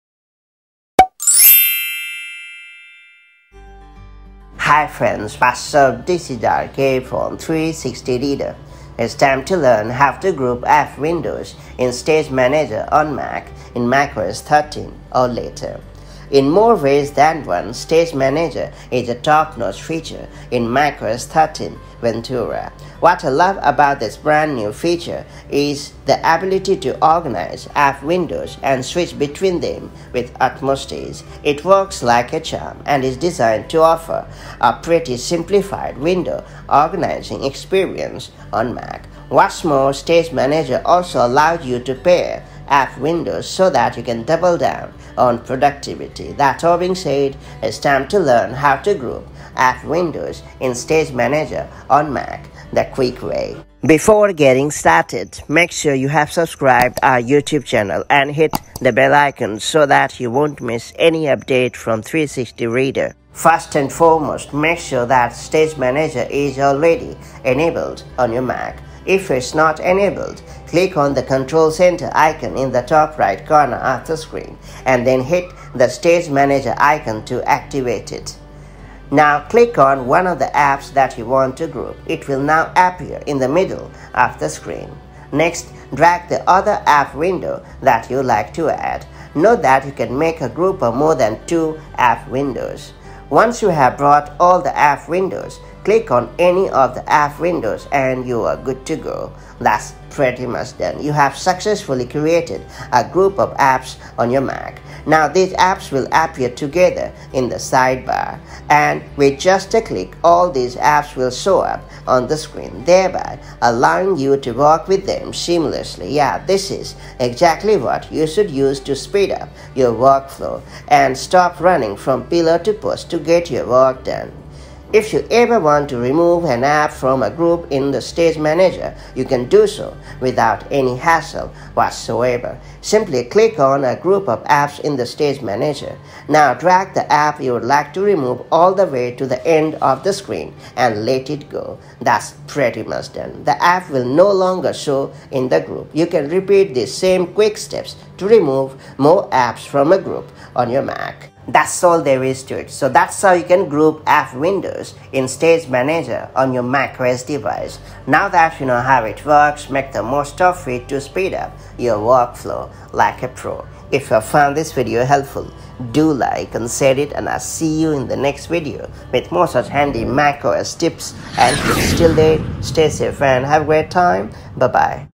How to Group App Windows in Stage Manager in macOS 13 Ventura on Mac 🔥
Here is how you can quickly pair app windows in Stage Manager in macOS 13 Ventura on your Mac to speed up the workflow. Let’s get started!